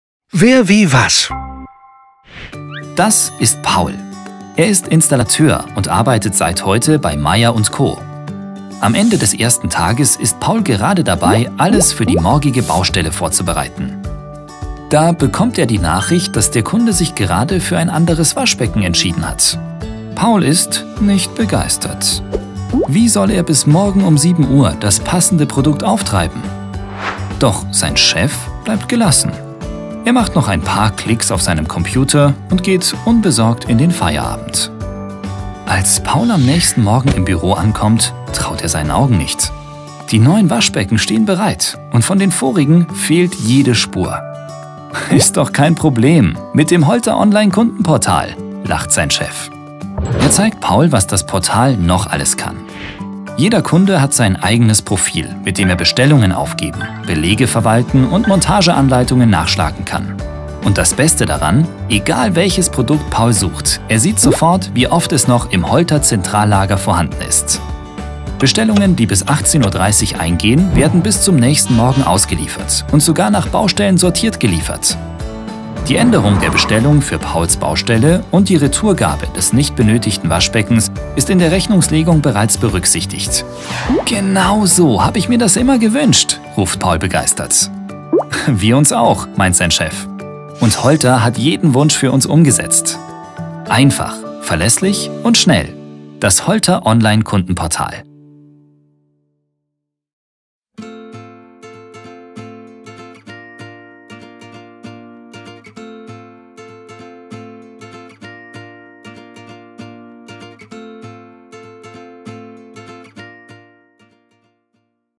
Holter - Online Kundenportal
Einfach, verlässlich und schnell!

Das Holter Online-Kundenportal erklärt von VerVieVas! Montageanleitungen online lesen, individuelle Kundenprofile oder die Verwaltung von Belegen: das Holter Online-Kundenportal kann all das und noch mehr!